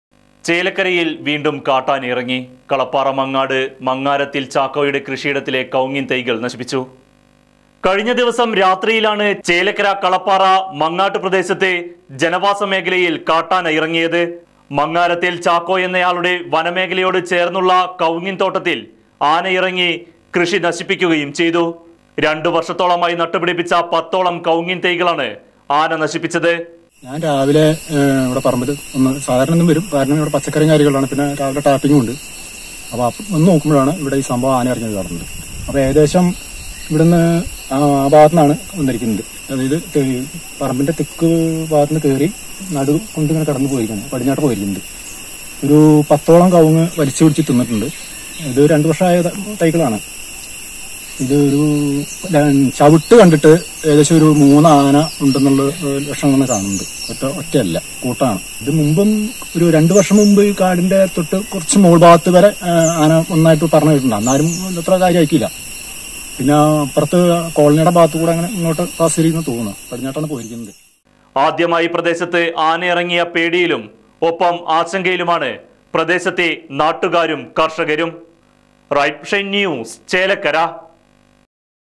ചേലക്കര::വീണ്ടും കാട്ടാനയിറങ്ങി; കളപ്പാറ ചാക്കോയുടെ കൃഷിയിടത്തിലെ കവുങ്ങിൻ തൈകൾ നശിപ്പിച്ചു
ചേലക്കര::വീണ്ടും കാട്ടാനയിറങ്ങി; കളപ്പാറ മങ്ങാട് മങ്ങാരത്തിൽ ചാക്കോയുടെ കൃഷിയിടത്തിലെ കവുങ്ങിൻ തൈകൾ നശിപ്പിച്ചു